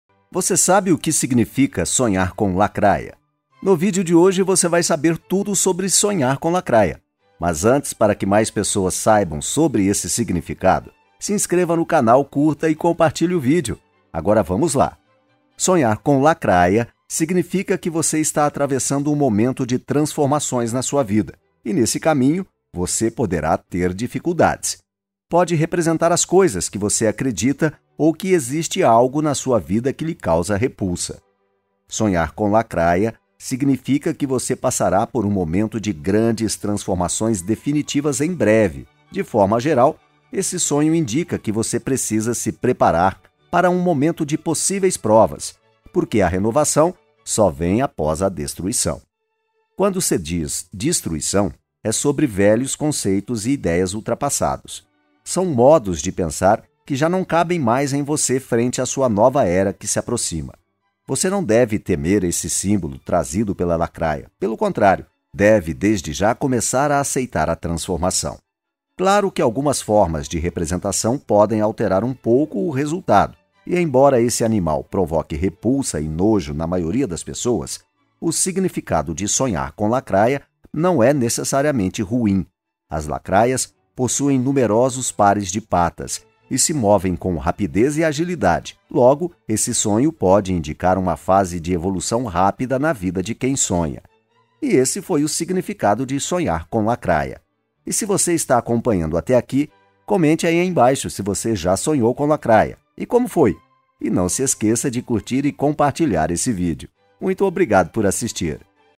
SONHAR COM LACRAIA? Saiba o significado de sonhar com lacraia atacando, morta ou grande
Veja o significado de sonhar com LACRAIA! Neste vídeo eu te explico tudo sobre sonhar com lacraia atacando, morta, ou grande.